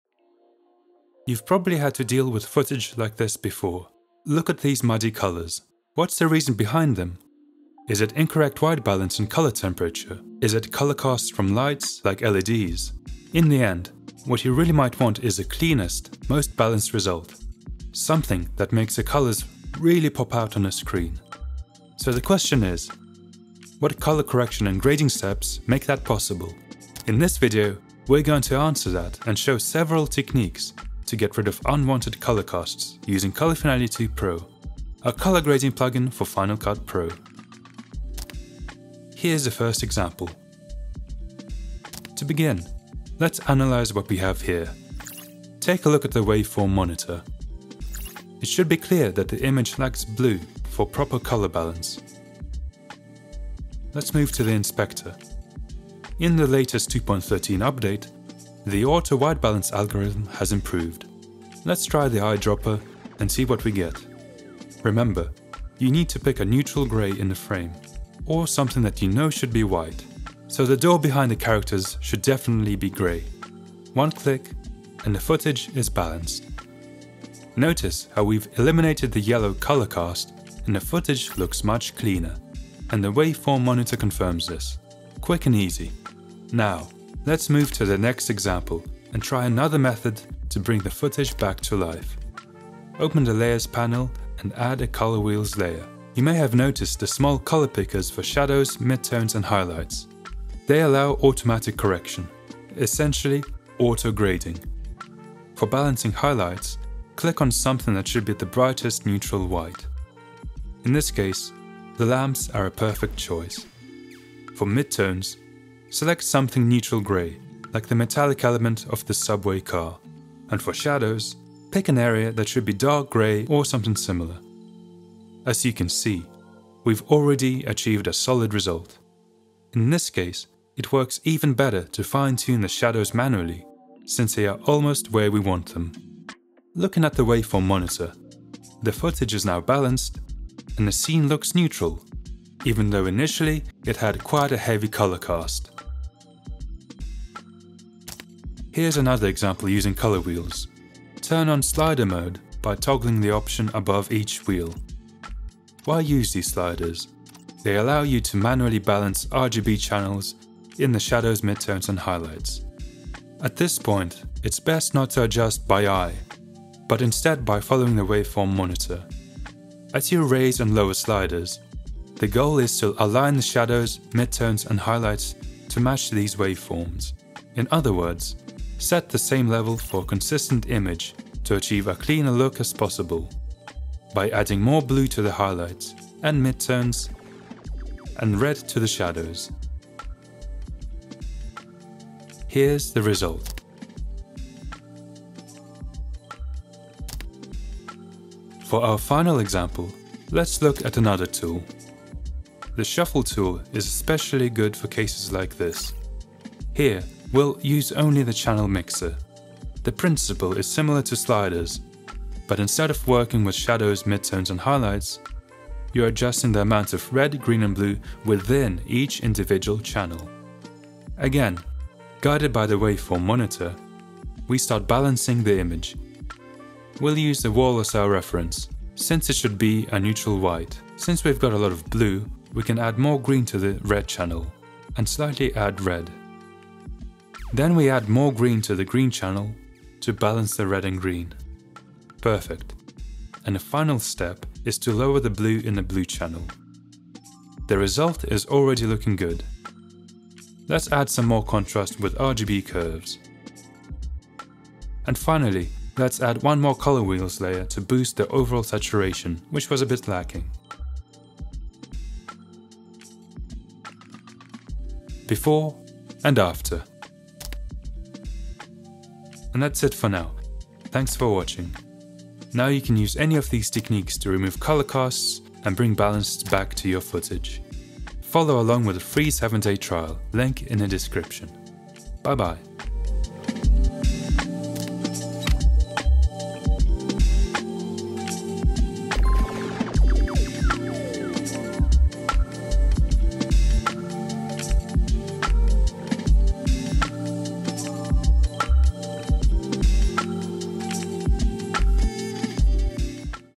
Vibrant colors every time with Color Finale 2 Pro and Final Cut Pro
Are you color grading footage that's been shot in less-than-ideal circumstances? In this video we take you through some simple but powerful methods of removing unwanted color casts in your footage.